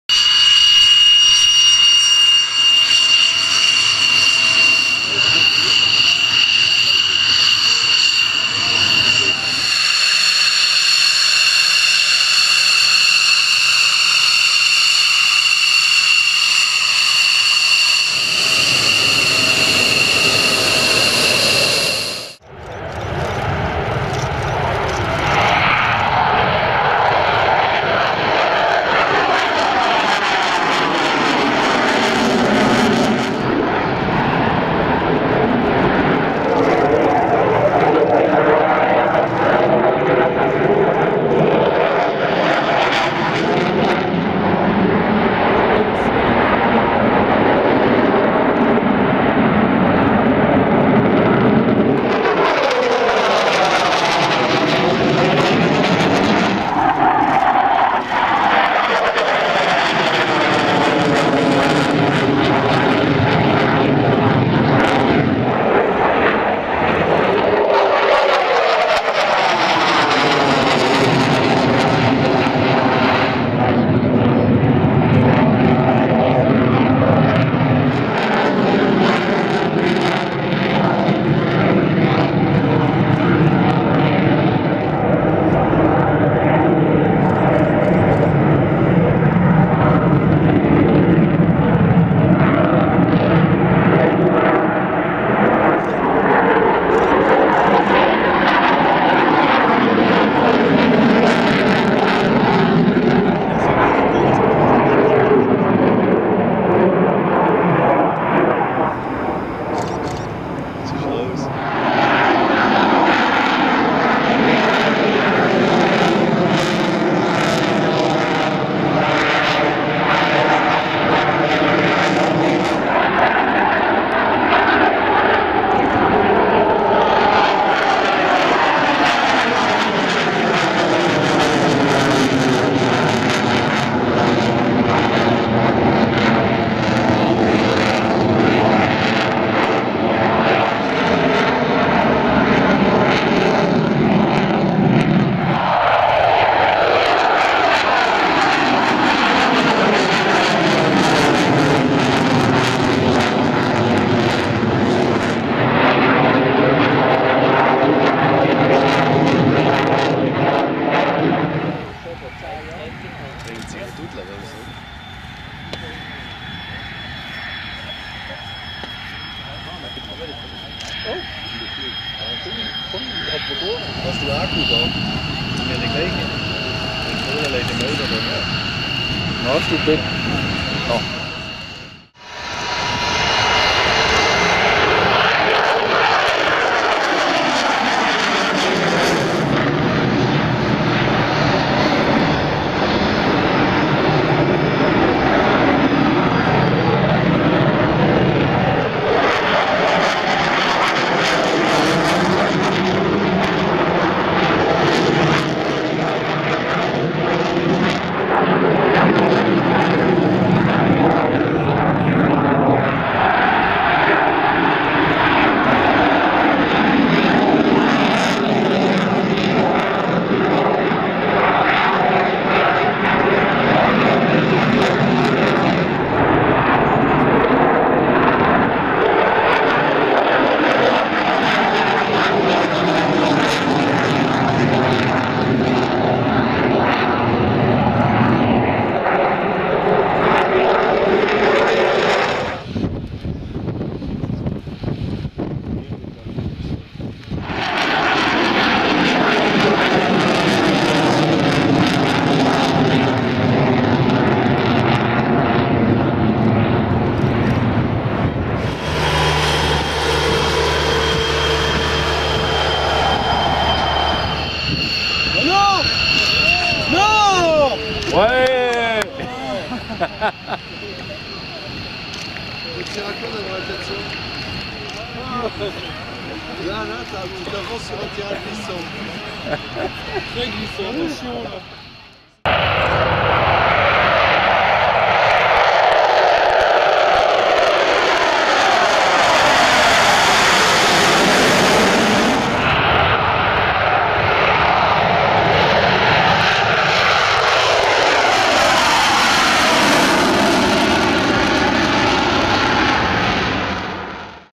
Air 14 Payerne Highlights F-16 Belgien
Highlights vom Arrival und dem Display der belgischen F-16 bei der Air 14 in Payerne